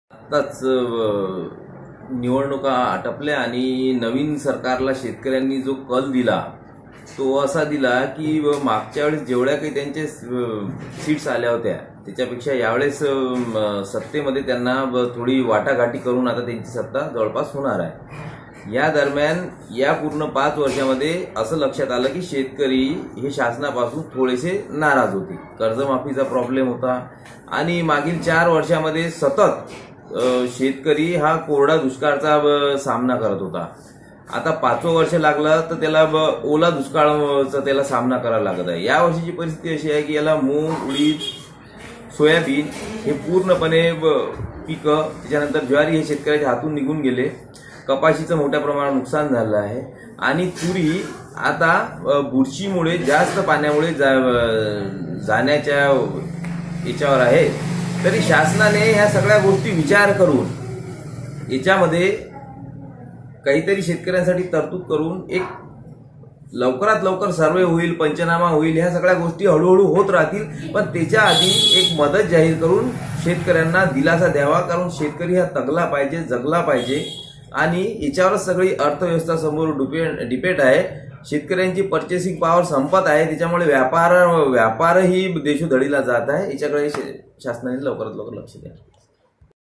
परतींच्या पावसाने झालेल्या सोयाबीन , ज्वारी ,कपाशी, कांदा, भाजीपाला यांचे मोठ्या प्रमाणात नुकसान
अकोला - निवडणूका झाल्या नवीन सरकारला जो शेतकऱ्यांनी कल दिला, आता पुन्हा त्यांची सता येणार आहे. पंरतु शेतकरी प्रशासनापासून नाराज आहे. तसेच ४ वर्षापासून कोरडा दुष्काळ होता, आता या ५ वर्षात ओला दुष्काळ आल्याने शेतकऱ्याला  ज्वारी ,कपाशी, कांदा, भाजीपाला यांचे मोठ्या प्रमाणात नुकसान व दुष्काळाचा सामना करावा लागत आहे. असे शेतकरी संघटना अकोला जिल्हा प्रमुख लक्ष्मीकांत कौठकार RNO शी म्हणाले